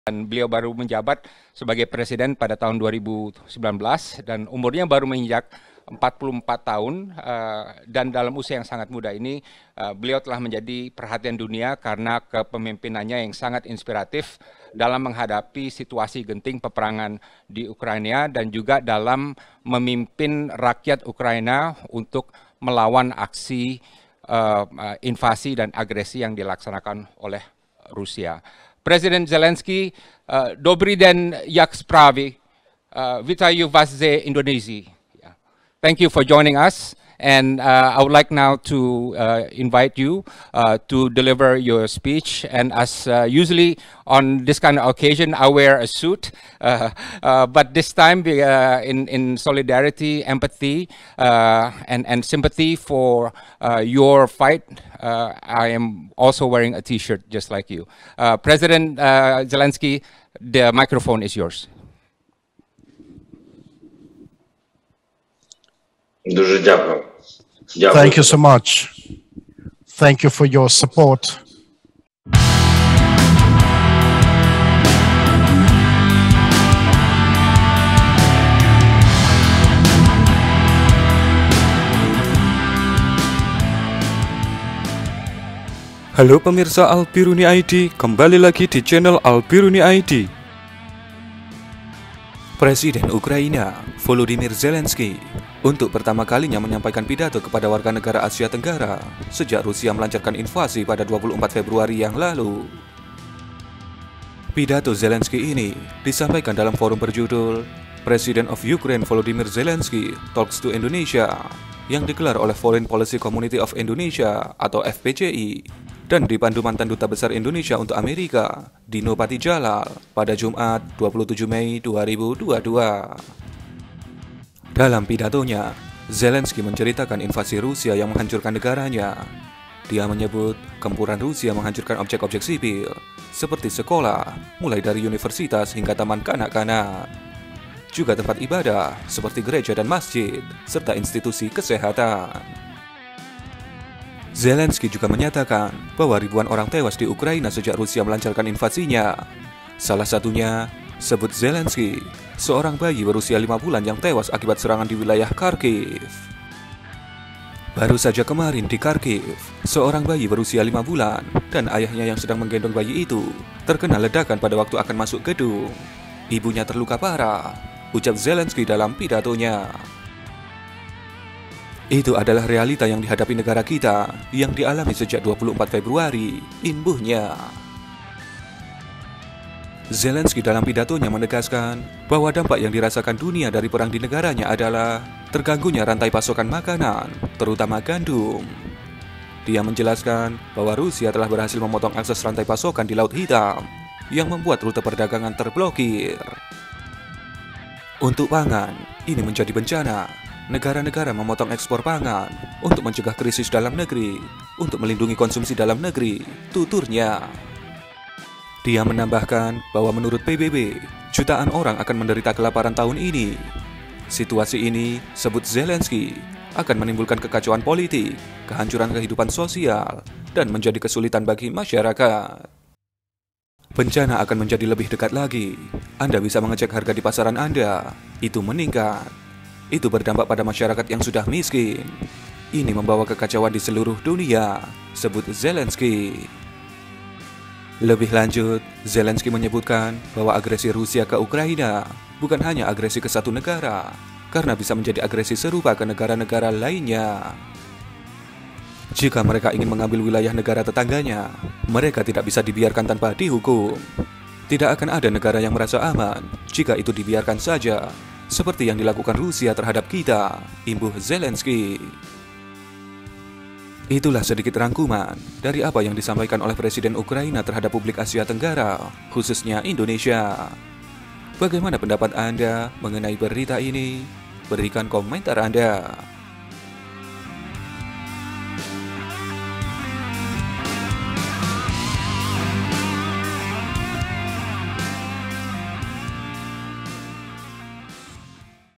Pertama Kali Presiden Zelensky Bicara Ke Publik Indonesia, Ini Yang Dia Sampaikan
Pada video kali ini, kita akan membahas mengenai curhatan dan juga permintaan dari presiden Ukraina mengenai apa yang saat ini terjadi di negaranya.